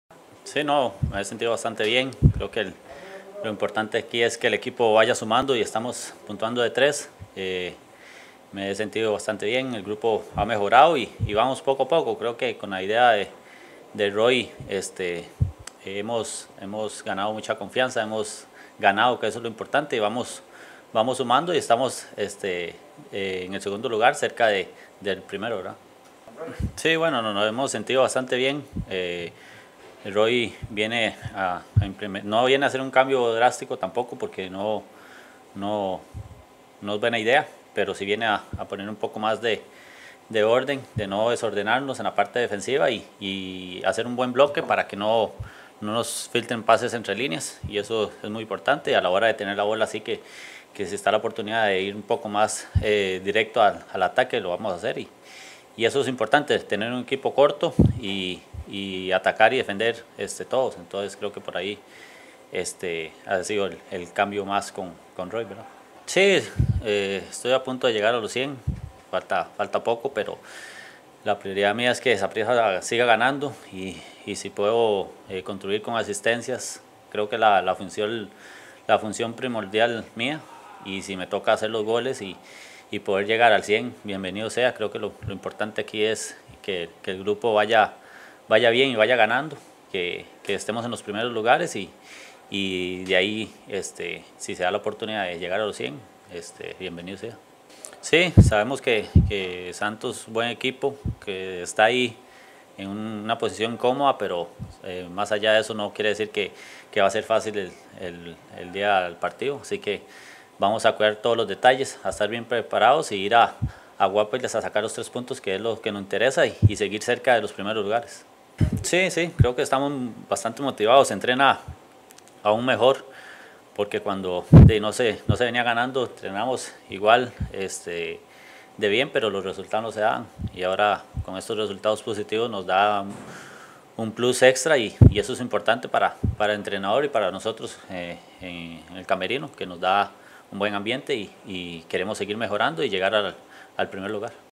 Marvin Ángulo en conferencia de prensa antes de la jornada 10 de LigaPromerica
Video: Prensa SAP